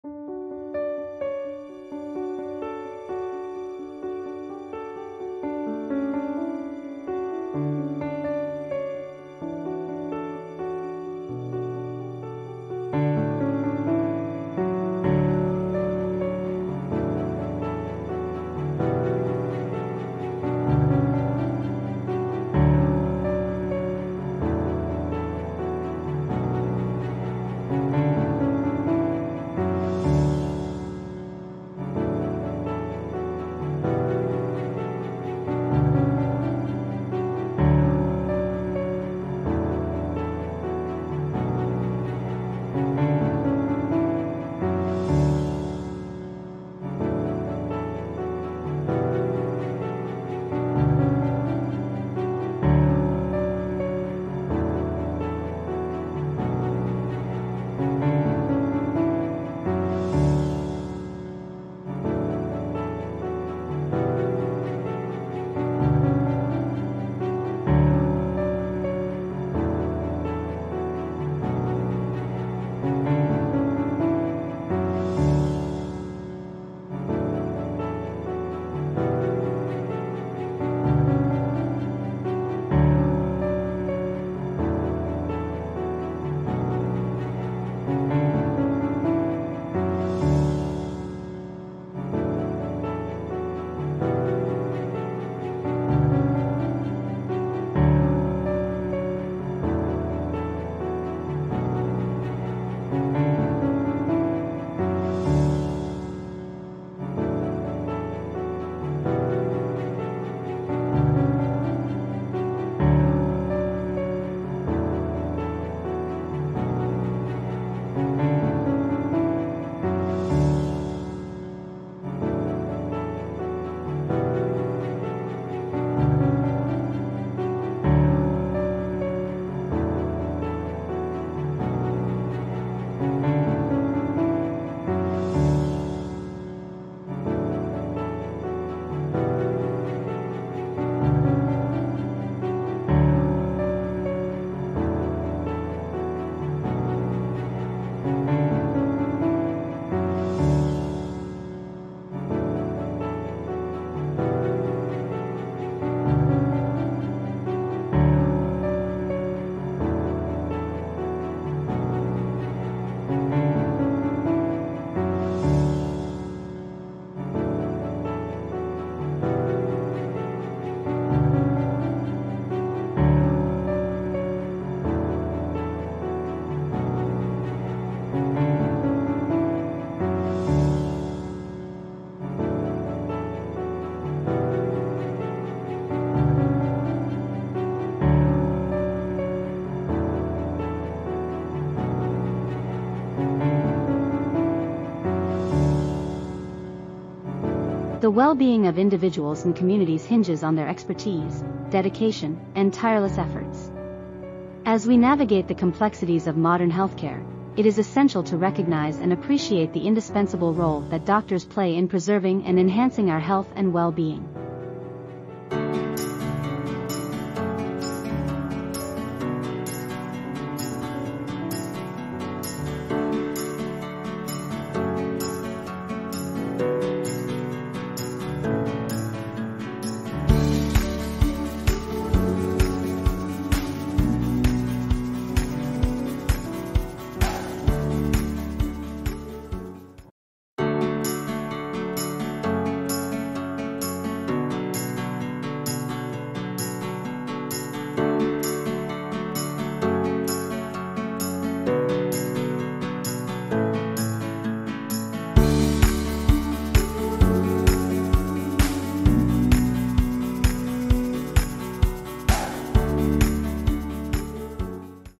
The Impact of Doctors on Healthcare (5 Minutes)
Doctors play a crucial role in the healthcare system and have a significant impact on patient care and overall healthcare outcomes. As highly trained medical professionals, doctors provide expertise, knowledge, and skills that are essential for diagnosing and treating various medical conditions. They are responsible for making critical decisions about patient care, prescribing medications, performing surgeries, and coordinating with other healthcare professionals. Doctors also serve as advocates for their patients, ensuring they receive the best possible care and treatment. Their expertise and experience contribute to improved healthcare outcomes, reduced mortality rates, and enhanced overall patient well-being. Additionally, doctors play a vital role in medical research, advancing medical knowledge, and developing new treatments and therapies. Their dedication, compassion, and commitment to the well-being of their patients make doctors invaluable contributors to the healthcare system.